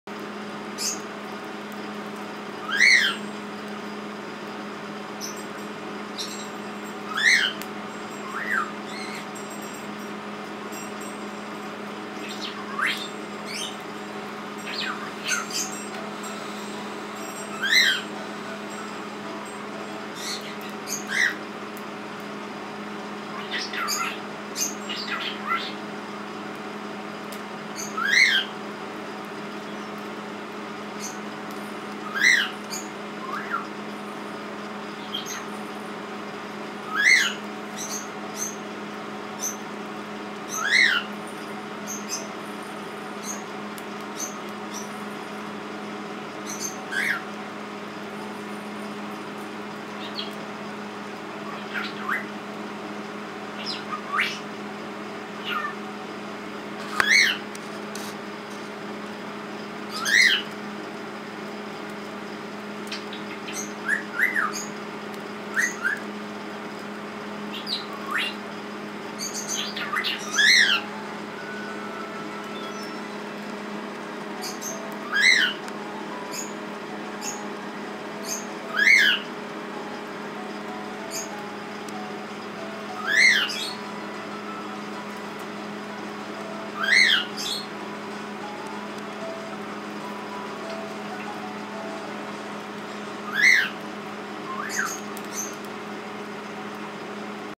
Mango and the Feather
Mango hanging out with the tiels, playing with one of  Dally's feathers